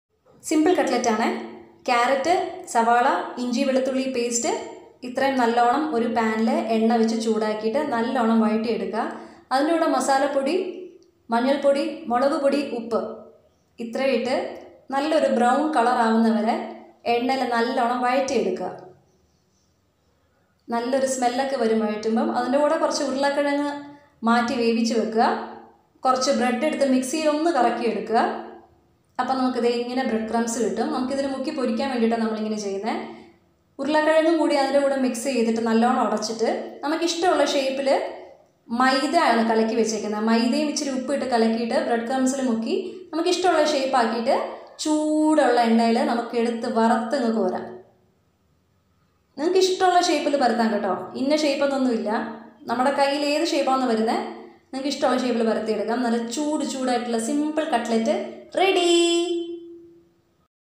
Cutlet simple version
Onion - 1
Carrot small 1
Potato medium size 2
Turmeric powder one tea spoon
Chilli powder one tea spoon
Garam masala one tea spoon
Maida 1 tablespoon
Bread crumps